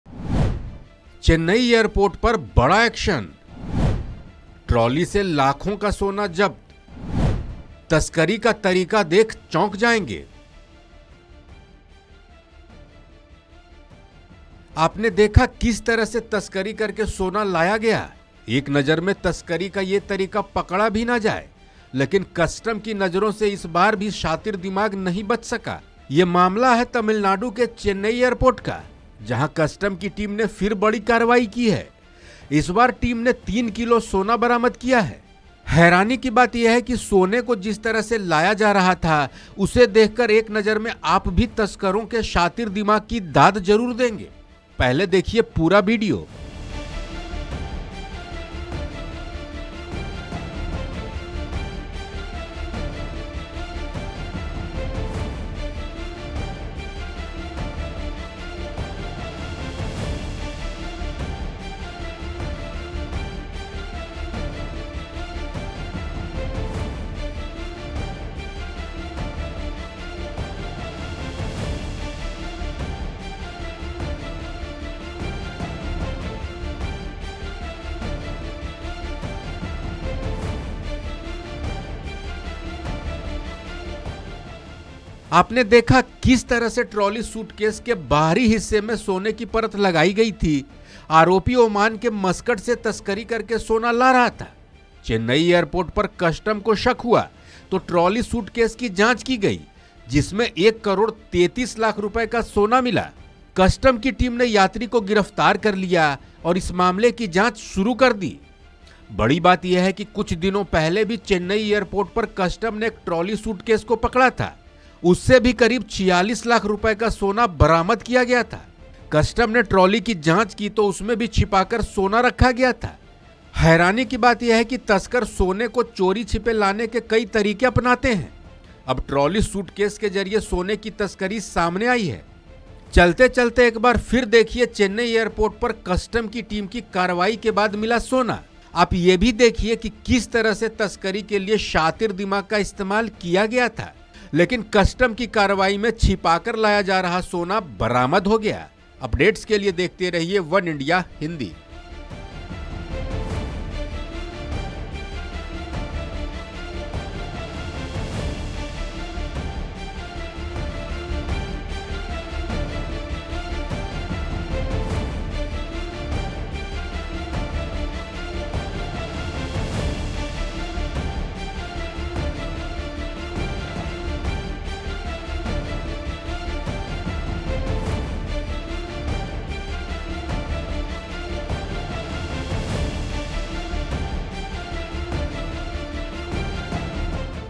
Chennai Airport पर Trolly Suitcase से 1.33 Crore का Gold बरामद, देखें वीडियो | वनइंडिया हिंदी *News
चेन्नई एयरपोर्ट (Chennai airport) पर कस्टम की टीम (Custom team) ने फिर बड़ी कार्रवाई की है. इस बार टीम ने तीन किलो सोना (3kg gold) बरामद किया है. आरोपी ओमान के मस्कट (Muscat) से तस्करी करके सोना ला रहा था. चेन्नई एयरपोर्ट पर कस्टम को शक हुआ तो ट्रॉली सूटकेस (trolly suitcase) की जांच की गई, जिसमें 1.33 करोड़ रुपए का सोना मिला.